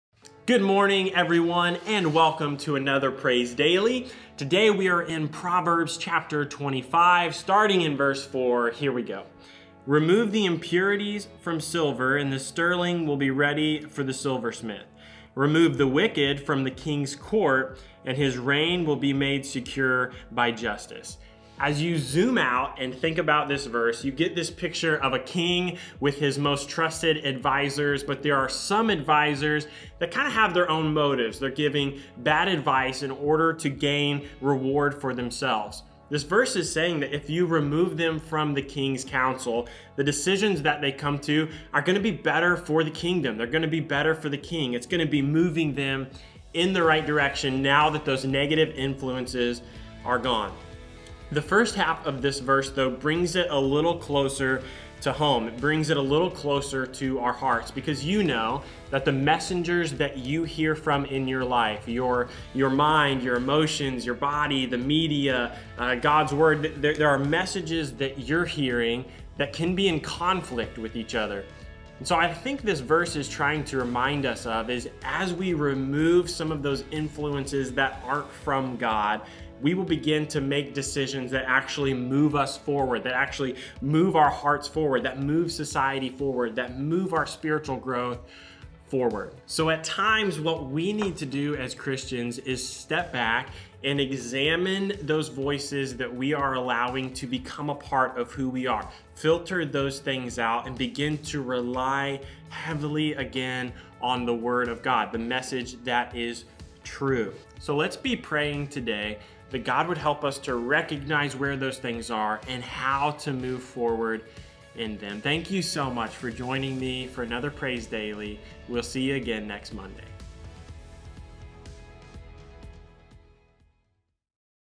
All The King's Men | Proverbs 25 | Nathan Mortenson
The voices we allow to impact our decision making can greatly determine where we end up. Just as a king chooses wisely who he will listen to, we must wisely make this choice as well.